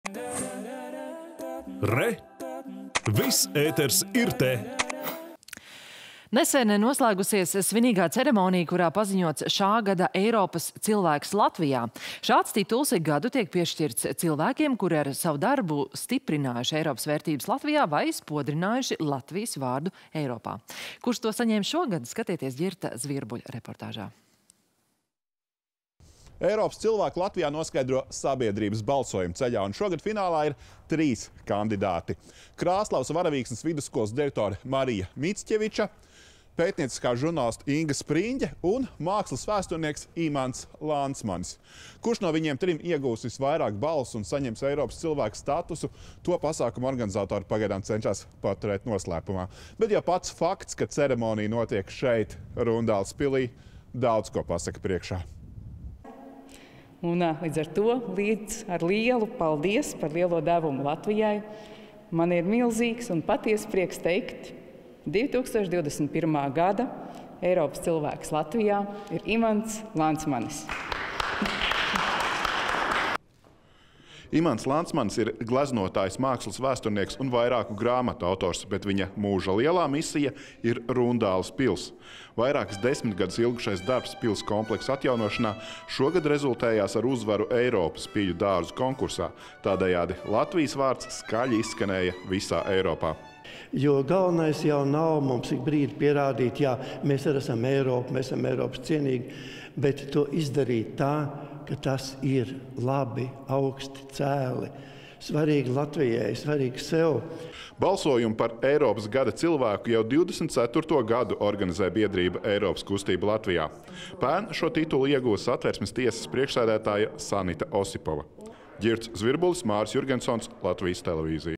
Noskaidrots 2021. gada Eiropas cilvēks Latvijā
Pirms neilga brīža noslēgusies svinīgā ceremonija, kurā paziņots šā gada Eiropas cilvēks Latvijā. Šāds tituls ik gadu tiek piešķirts cilvēkiem, kuri ar savu darbu stiprinājuši Eiropas vērtības Latvijā vai spodrinājuši Latvijas vārdu Eiropā. Kurš to saņēmis šogad – skatieties Ģirta Zvirbuļa reportāžā.